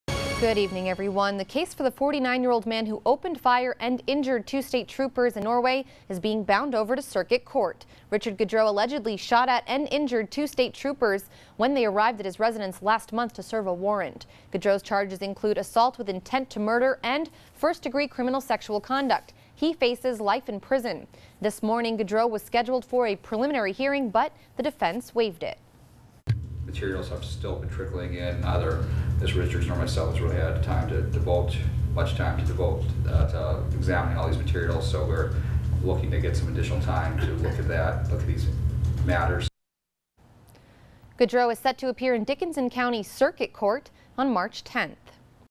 Norway Shooting Case Bound Over
The case for the 49-year old man who opened fire injuring two state troopers in Norway is being bound over to Circuit Court.